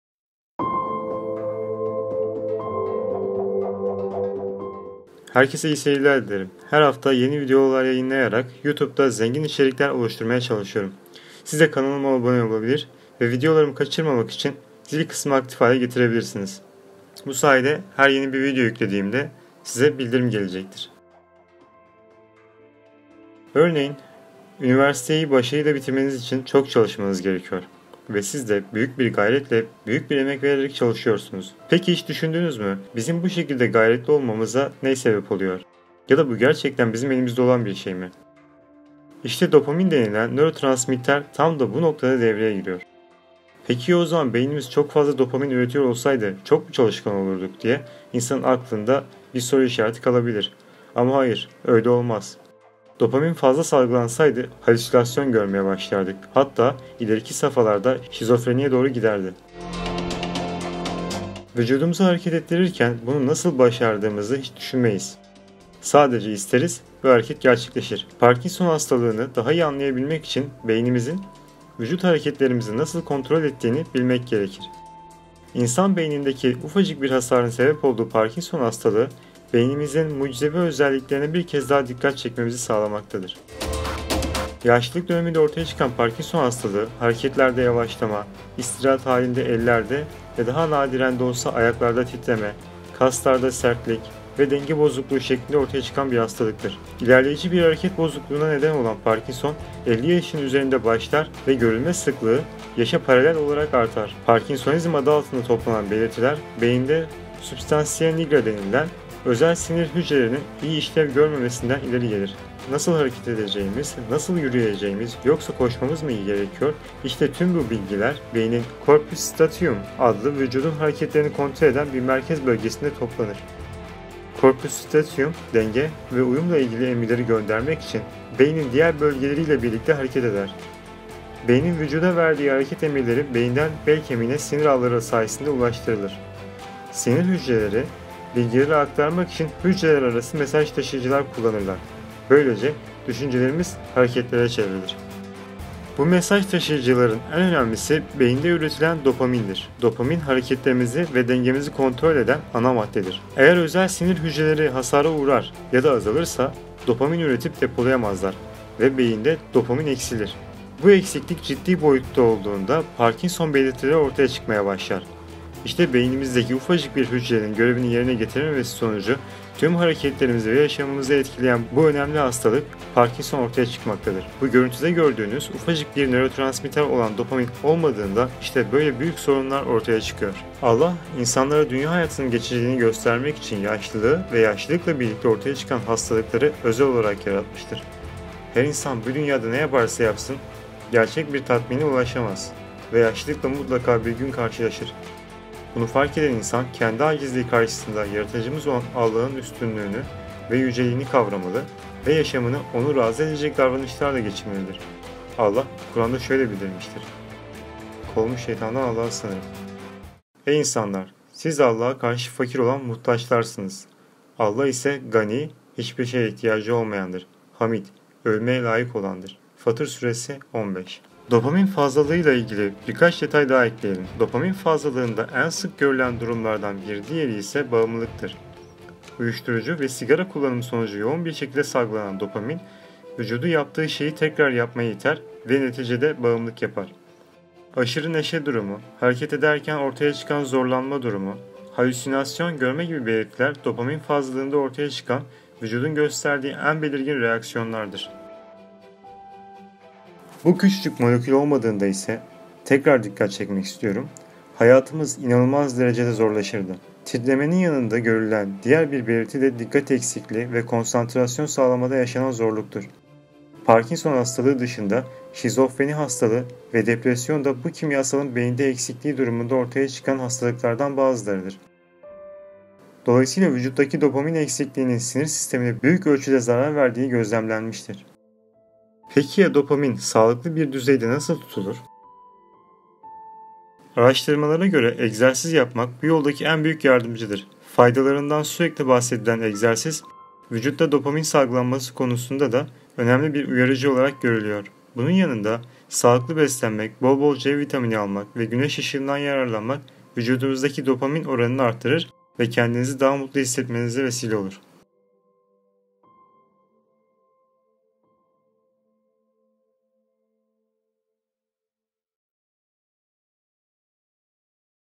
Sizi Çalıştıran Aslında Çok Küçük Bir Molekül Dopamin (Parkinson Hastalığı)
Yorumlarınız ve beğenileriniz önemli
Herkese iyi seyirler dilerim her hafta yeni videolar yayınlayarak YouTube’da zengin içerikler oluşturmaya çalışıyorum.
Örneğin üniversiteyi başarıyla bitirmeniz için çok çalışmanız gerekiyor ve sizde büyük bir gayretle, büyük bir emek vererek çalışıyorsunuz. Peki hiç düşündünüz mü bizim bu şekilde gayretli olmamıza ney sebep oluyor. Ya da bu gerçekten bizim elimizde olan birşey mi? İşte dopamin denilen nörotransmitter tam da bu noktada devreye giriyor.
Peki ya o zaman beynimiz çok fazla dopamin üretiyor olsaydı çok mu çalışkan olurduk diye insanın aklında bir soru işareti kalabilir ama hayır öyle olmaz dopamin fazla salgılansaydı halisülasyon görmeye başlardık, hatta ileriki safalarda şizofreniye doğru giderdi.
Vücudumuzu hareket ettirirken bunu nasıl başardığımızı hiç düşünmeyiz. Sadece isteriz ve hareket gerçekleşir. Parkinson hastalığını daha iyi anlayabilmek için beynimizin vücut hareketlerimizi nasıl kontrol ettiğini bilmek gerekir.
İnsan beynindeki ufacık bir hasarın sebep olduğu parkinson hastalığı, beynimizin mucizevi özelliklerine bir kez daha dikkat çekmemizi sağlamaktadır.
Yaşlılık döneminde ortaya çıkan Parkinson hastalığı, hareketlerde yavaşlama, istirahat halinde ellerde ve daha nadiren de olsa ayaklarda titreme, kaslarda sertlik ve denge bozukluğu şeklinde ortaya çıkan bir hastalıktır. İlerleyici bir hareket bozukluğuna neden olan Parkinson, 50 yaşın üzerinde başlar ve görülme sıklığı yaşa paralel olarak artar. Parkinsonizm adı altında toplanan belirtiler beyinde "substansiya nigra" denilen özel sinir hücrelerinin iyi işlev görememesinden ileri gelir.
Nasıl hareket edeceğimiz, nasıl yürüyeceğimiz, yoksa koşmamız mı gerekiyor işte tüm bu bilgiler beynin "korpüs stratium" adlı, vücudun hareketlerini kontrol eden bir merkez bölgesinde toplanır. Korpüs stratium denge ve uyumla ilgili emirleri göndermek için beynin diğer bölgeleriyle birlikte hareket eder. Beynin vücuda verdiği hareket emirleri beyinden belkemiğine sinir ağları sayesinde ulaştırılır. Sinir hücreleri bilgileri aktarmak için hücrelerarası mesaj taşıyıcılar kullanırlar. Böylece düşüncelerimiz hareketlere çevrilir.

"Ey insanlar, siz Allah'a (karşı fakir olan) muhtaçlarsınız. Allah ise Ganiy (hiçbir şeye ihtiyacı olmayan)dır. Hamid (övülmeye layık)tır." (Fatır Suresi, 15)

Dopmamin fazlalığıyla ilgili birkaç detay daha ekleyelim
Dopamin fazlalığında en sık görülen durumlardan bir diğeri ise bağımlılıktır. Uyuşturucu ve sigara kullanımı sonucu yoğun bir şekilde salgılanan dopamin, vücudu yaptığı şeyi tekrar yapmaya iter ve neticede bağımlılık yapar. Aşırı neşe durumu, hareket ederken ortaya çıkan zorlanma durumu, halüsinasyon görme gibi belirtiler dopamin fazlalığında ortaya çıkan vücudun gösterdiği en belirgin reaksiyonlardır.
Bu küçükük molekül olmadığında ise ( tekrar dikkat çekmek istiyorum) hayatımız inanılmaz derecede zorlaşırdı
Titremenin yanında görülen diğer bir belirti de dikkat eksikliği ve konsantrasyon sağlamada yaşanan zorluktur. Parkinson hastalığı dışında şizofreni hastalığı ve depresyon da bu kimyasalın beyinde eksikliği durumunda ortaya çıkan hastalıklardan bazılarıdır. Dolayısıyla vücuttaki dopamin eksikliğinin sinir sistemine büyük ölçüde zarar verdiğini gözlemlenmiştir.
Dopamin sağlıklı bir düzeyde nasıl tutulur?
Peki, vücudunuzdaki dopamin oranını aşırıya kaçmadan doğal yöntemlerle nasıl arttırabilirsiniz? Araştırmalara göre egzersiz yapmak bu yoldaki en büyük yardımcıdır. Faydalarından sürekli bahsedilen egzersiz, vücutta dopamin salgılanması konusunda da önemli bir uyarıcı olarak görülüyor. Bunun yanında sağlıklı beslenmek, bol bol C vitamini almak ve güneş ışığından yararlanmak vücudunuzdaki dopamin oranını arttırır ve kendinizi daha mutlu hissetmenizi sağlar.
toksoplazma virüsü
parkinson alzheimer
parkinson derneği